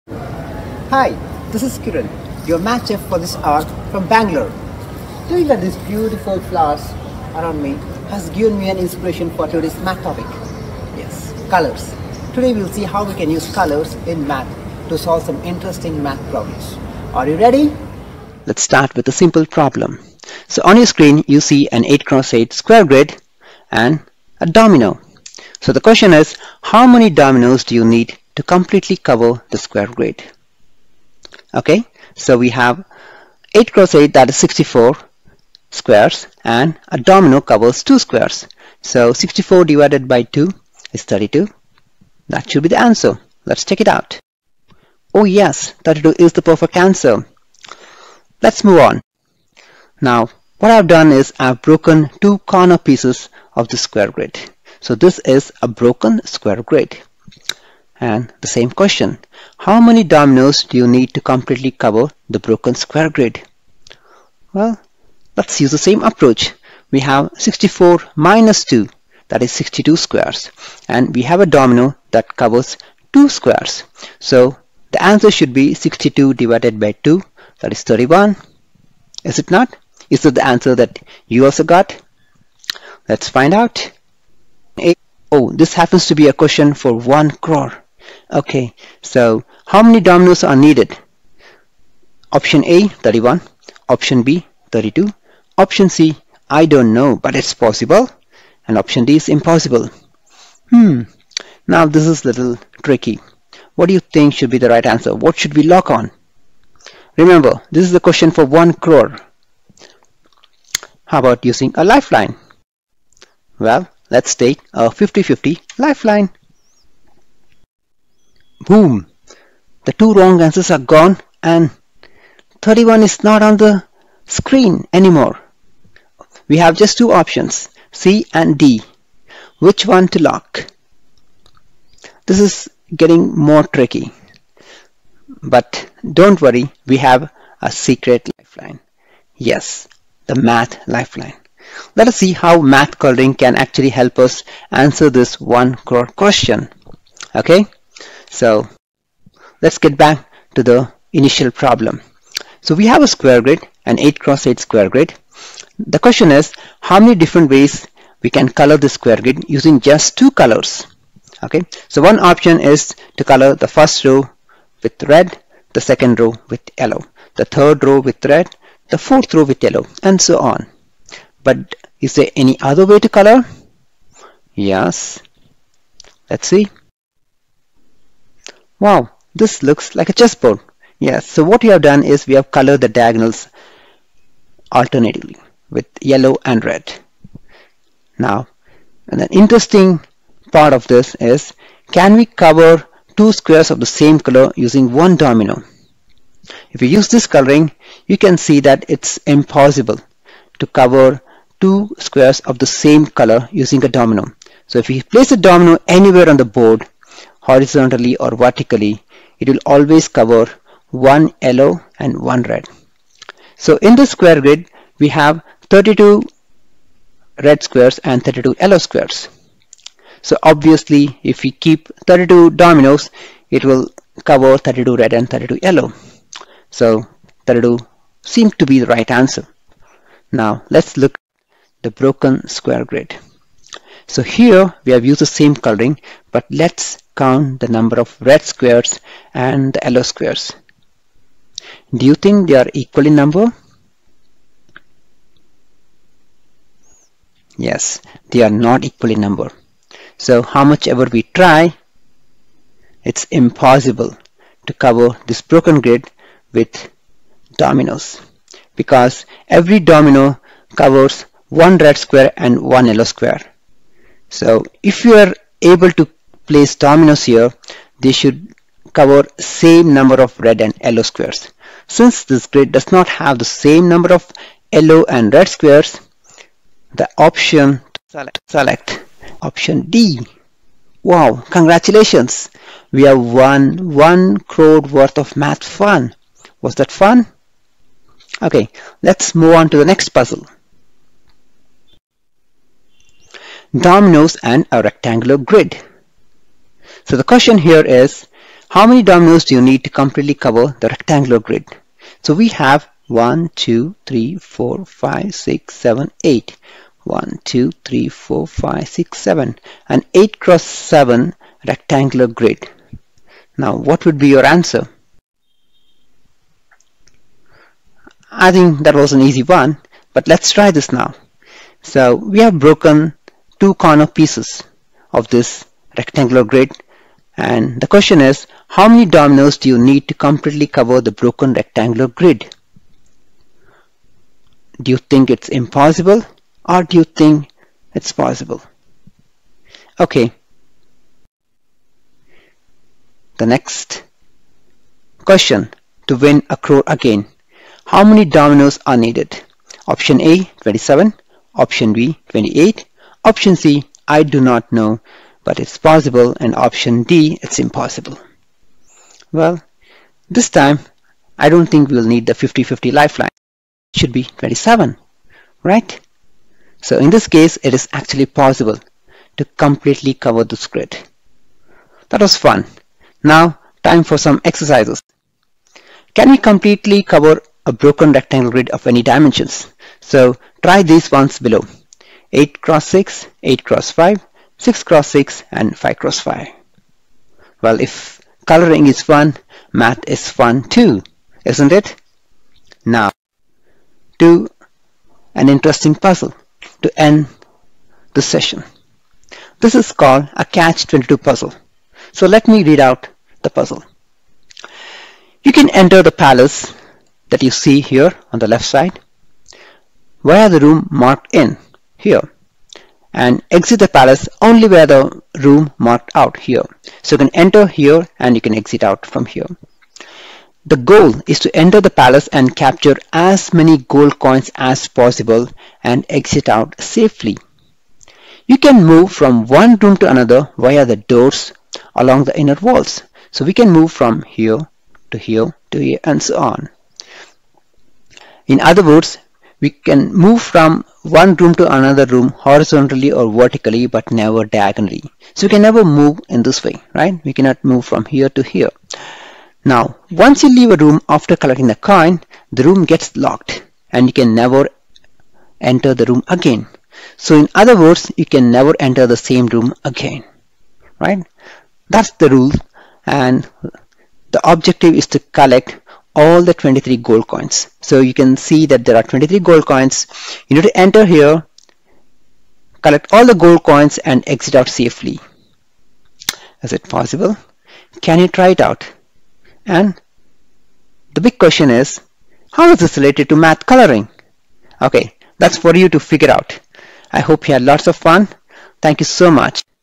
Google interview question| Fun Math with colouring!
🌌Famous mathematician G.H.Hardy once said:
"A mathematician, like a painter or a poet, is a maker of patterns. If his patterns are more permanent than theirs, it is because they are made with ideas. [...] The mathematician's patterns, like the painter's or the poet's, must be beautiful; the ideas like the colours or the words must fit together in a harmonious way."

🌈This video features Mr Kiran Bacche, co-founder at DhiMath and an author of international best-selling books where he will take you on an adventurous journey to explore patterns using COLOURS.
🙆‍♀️🙆Are you intrigued?
💬Watch the video and tell us your answers to the final riddle in the comments below.

⭐This video was a part of 24 H🌏ur Math Buffet organized by Julia Robinson Mathematics Festival.

⚠️WARNING: This will make you fall in love with mathematics!
Don't tell us we didn't warn you 😉